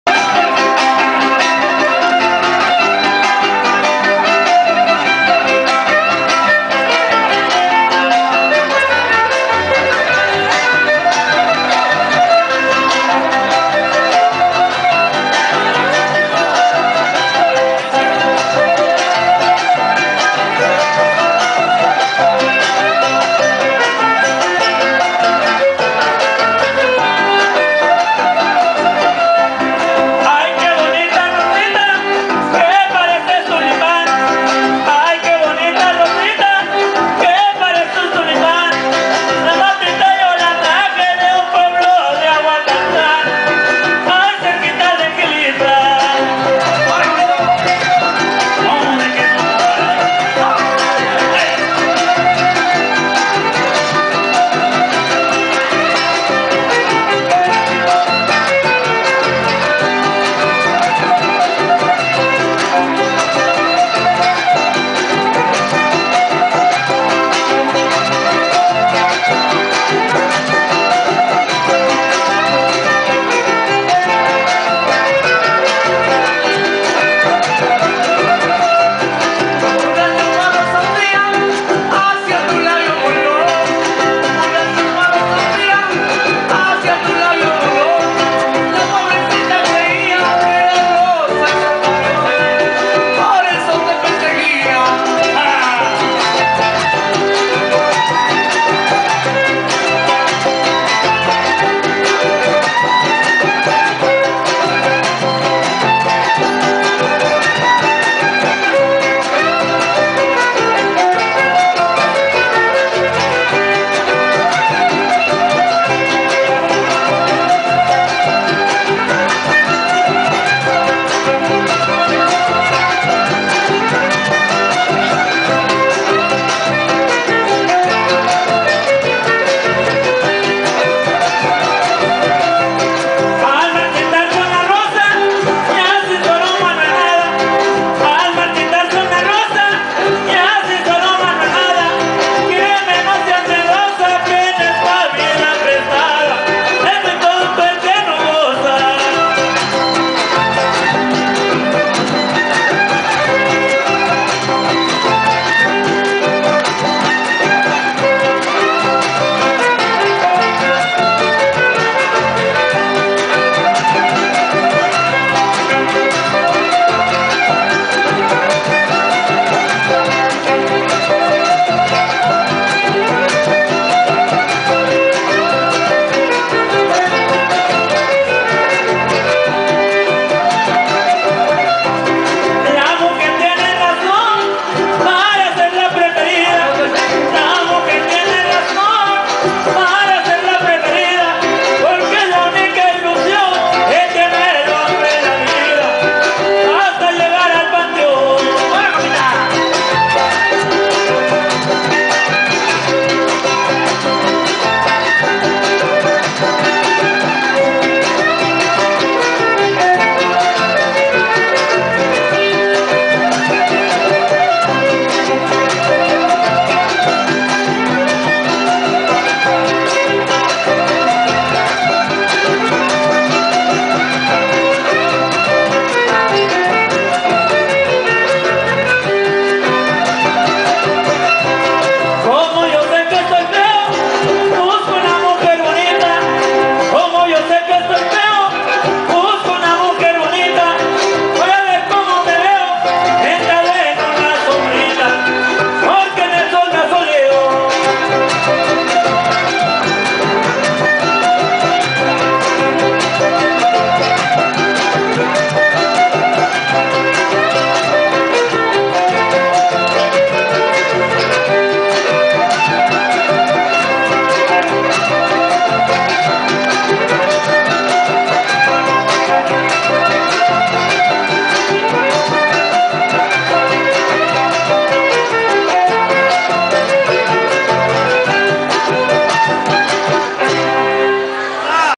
HUAPANGO
AQUI DISFRUTANDO DEL TRIO DE HUAPANGUEROS.